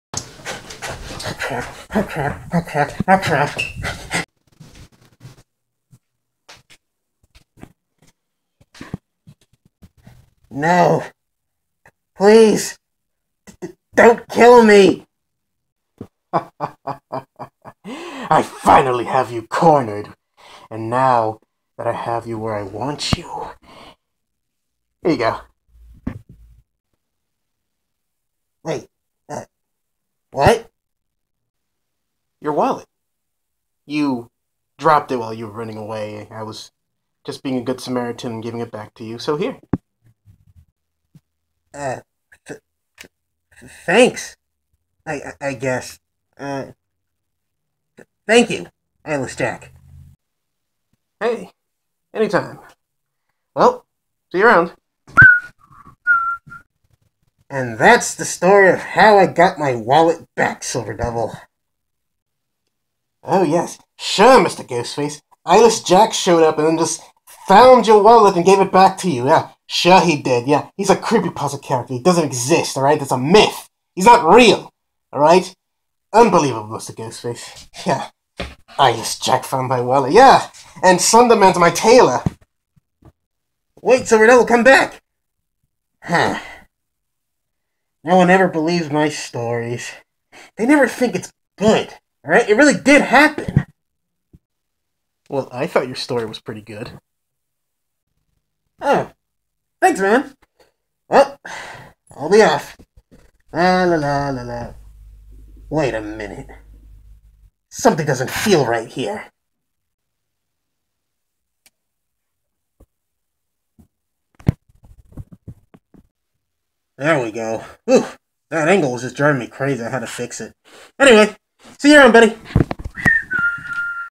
eyeless Jack
Mr. Ghost Face tells the story of the time he lost his wallet